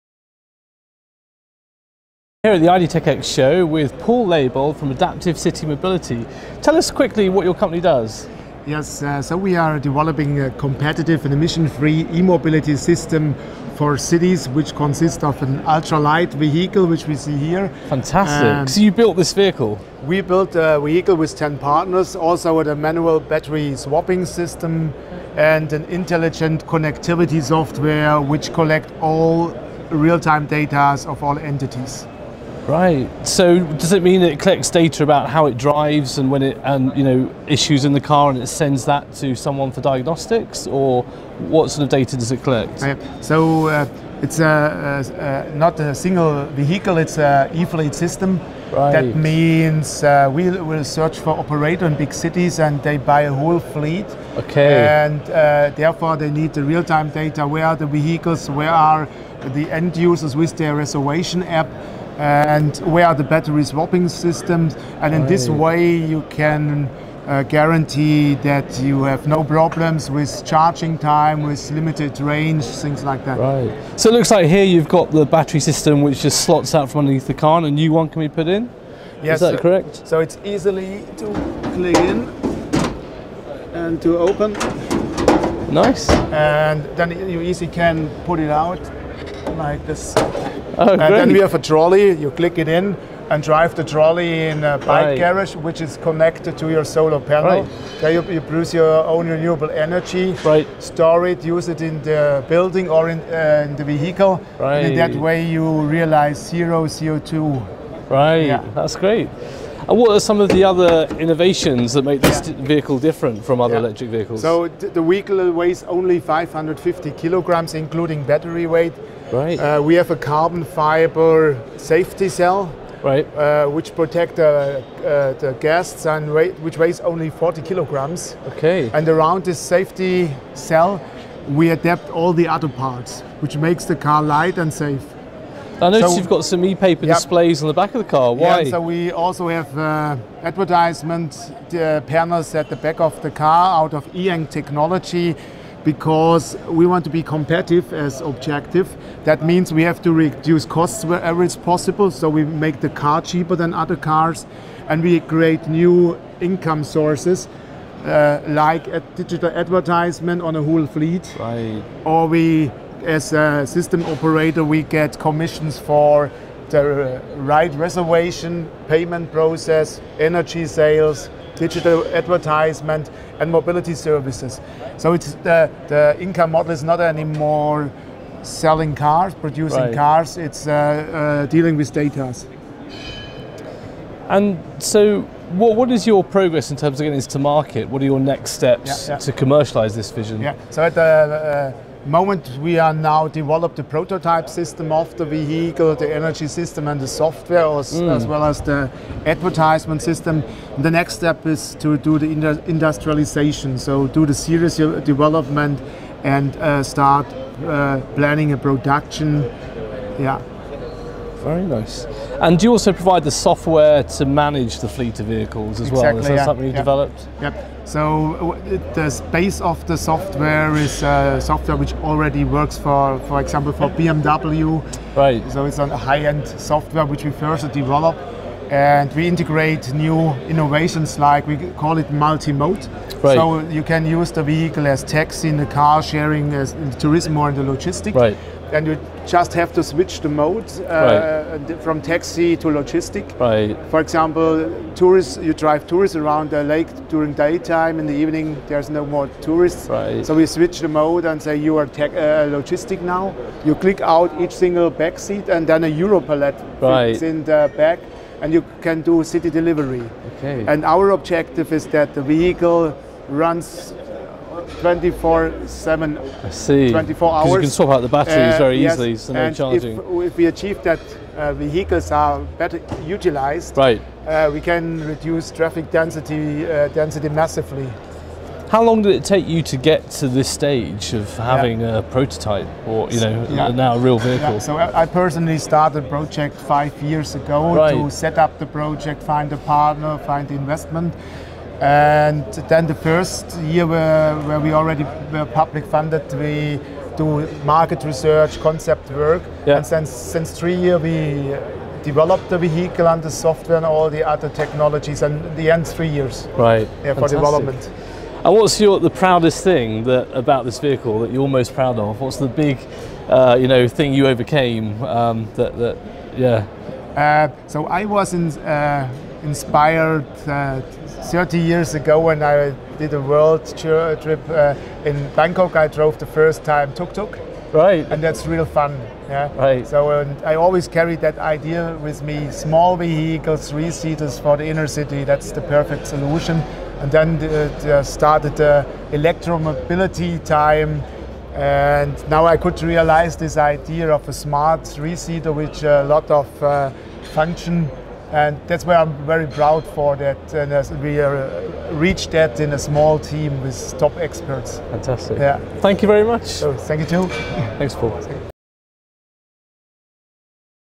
Emission-Free Electric Vehicle Prototype
The purpose of the "Adaptive City Mobility“ project is the development of a complete, emission-free mobility concept for cities. Ten cooperating technology ventures are constructing a small electric light-weight vehicle of the vehicle category L7E and developing it for series production. Combined with an innovative software, hardware and battery exchange system it depicts an extensive solution for the urban mobility of the future.

Filmed at the IDTechEx Show!